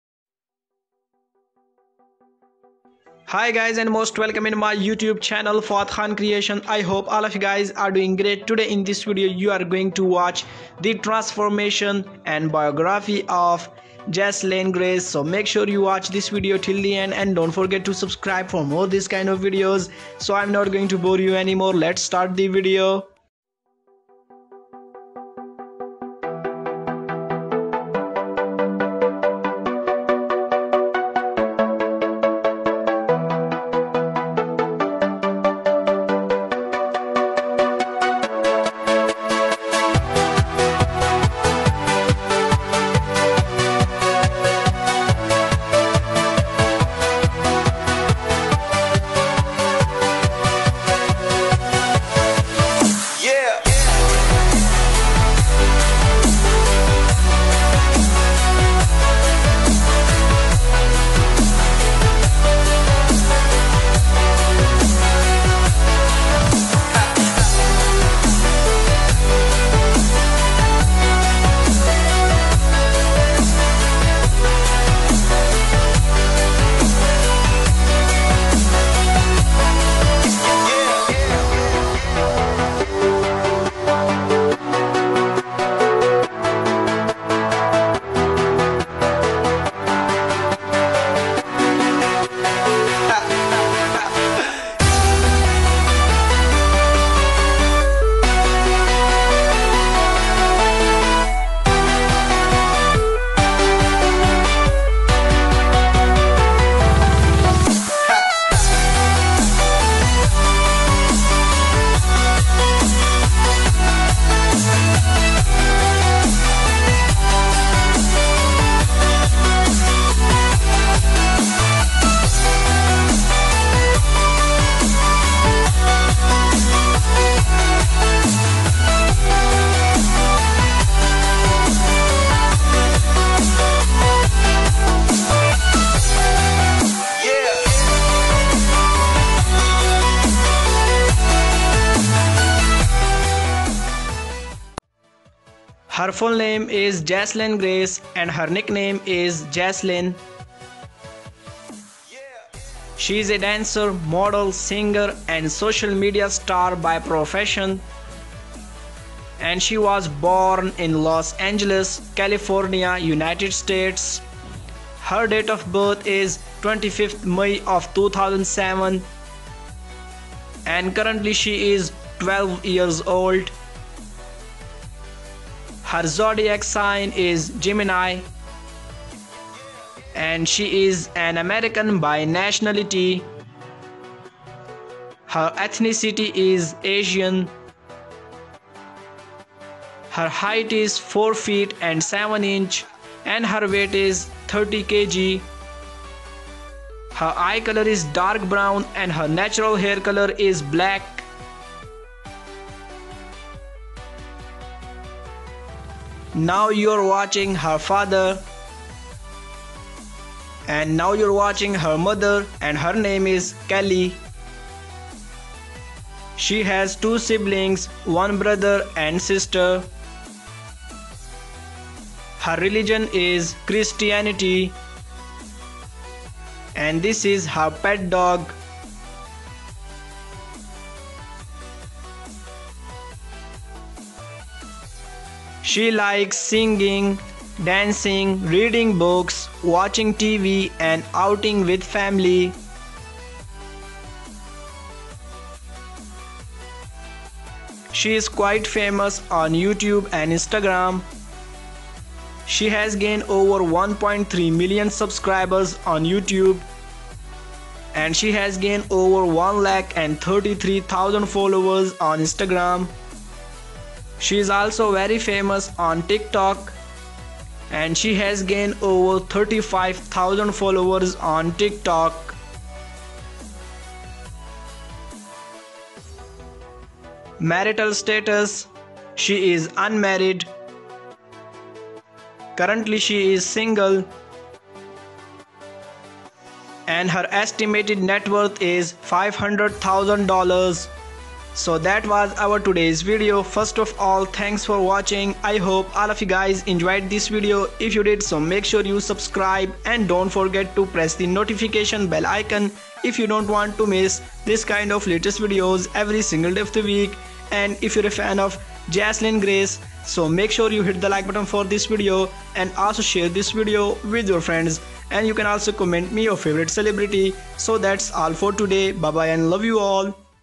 Jessalyn Grace Transformations from 01 to 12 Years | Family | Biography | Lifestyle | FK creation
THE MOST POPULAR AND VERY TALENTED AN AMERICAN SINGER, DANCER, MODEL AND SOCIAL MEDIA PERSONALITY JESSALYN GRACE 'S TRANSFORMATIONS AND LIFESTYLE AND BIOGRAPHY VIDEO BY FK CREATION 2020